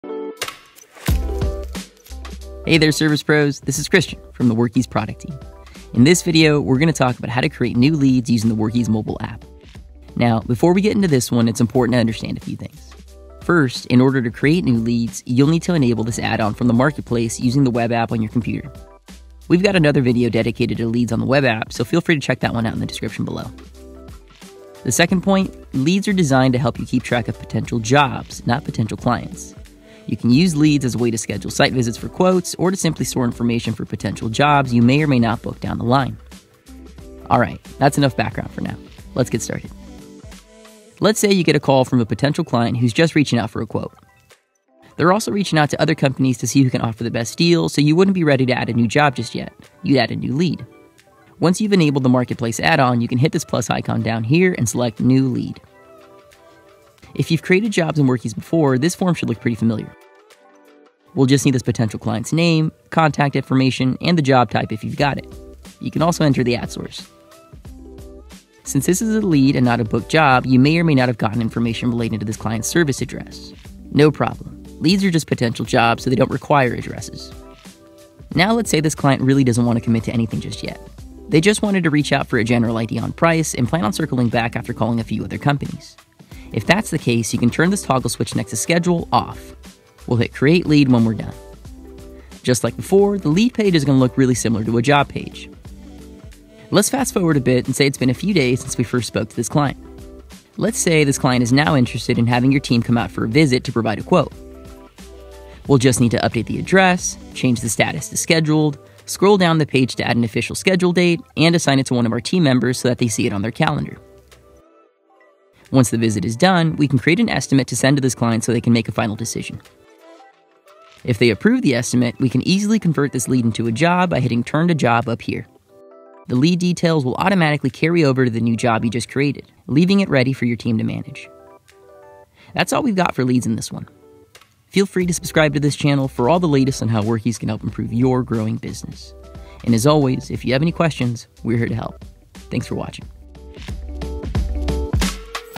Creating leads on the Workiz mobile app
Learn how to create leads using the Workiz mobile app